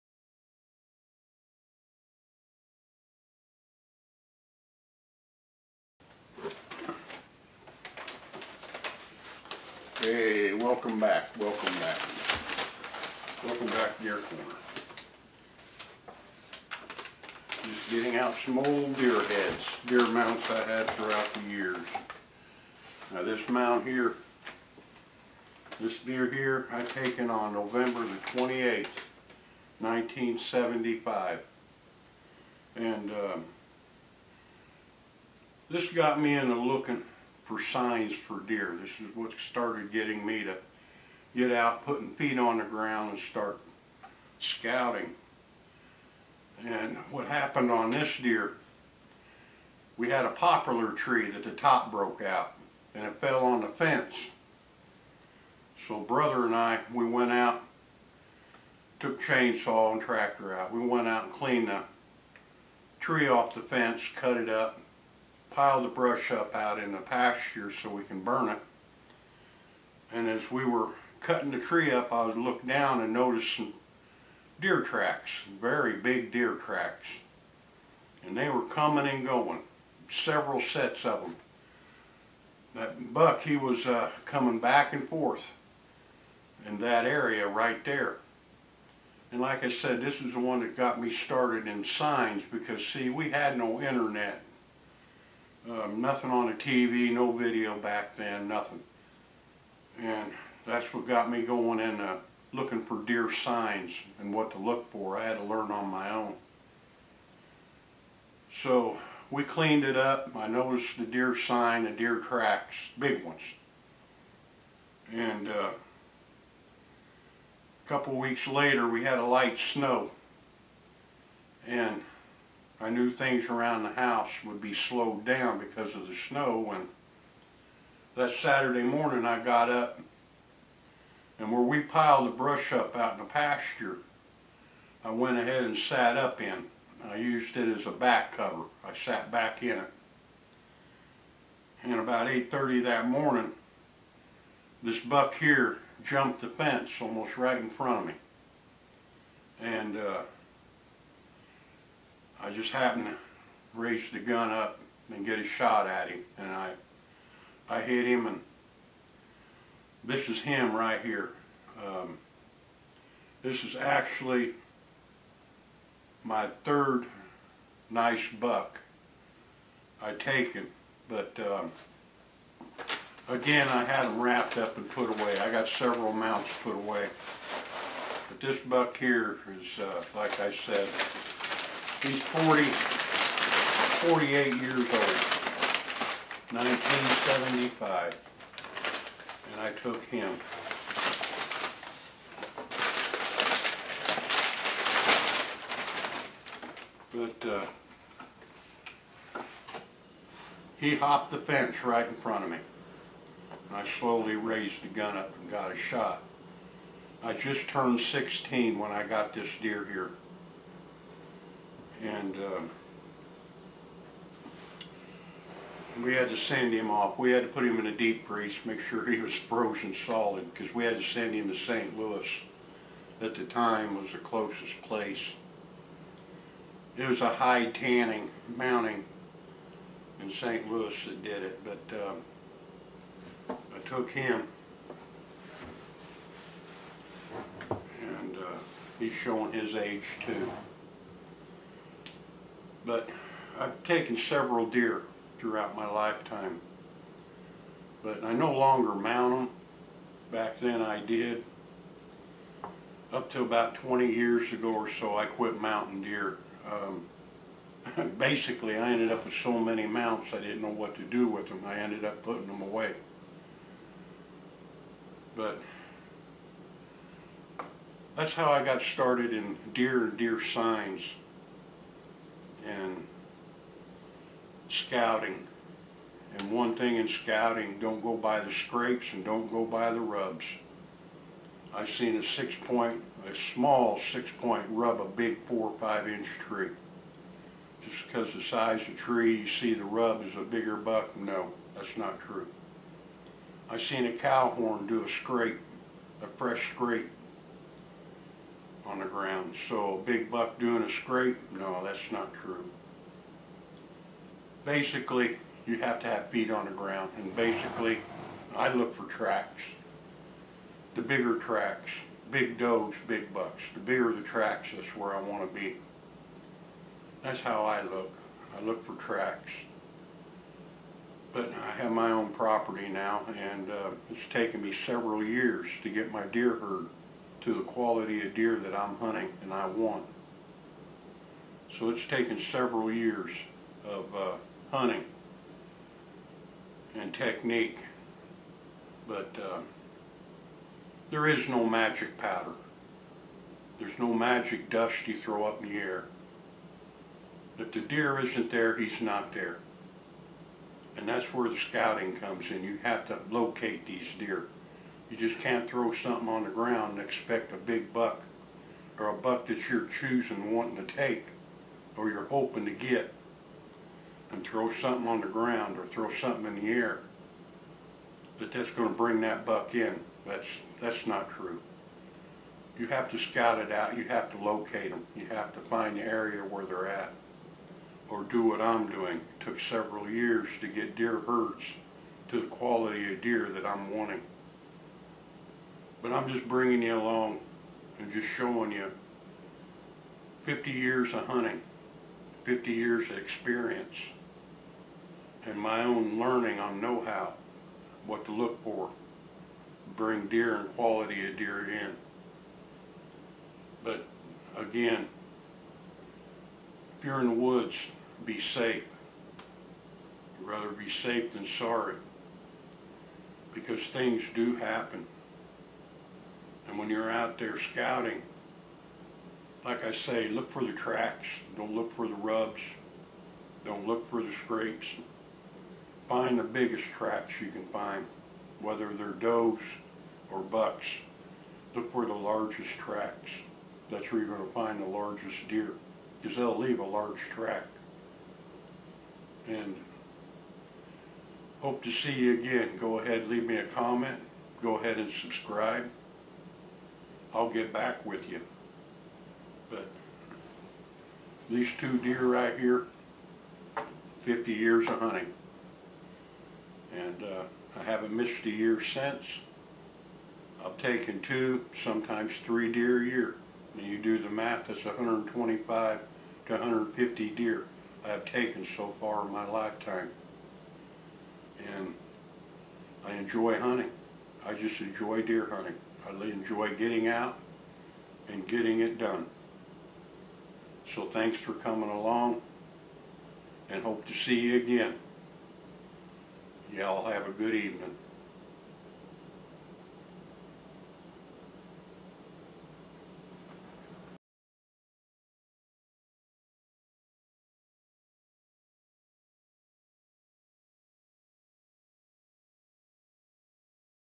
DEER HUNTING , 50 YEARS OF DEER HUNTING EXPERIENCE
Another look at DEER hunting experience for over 50 years and still keeping it simple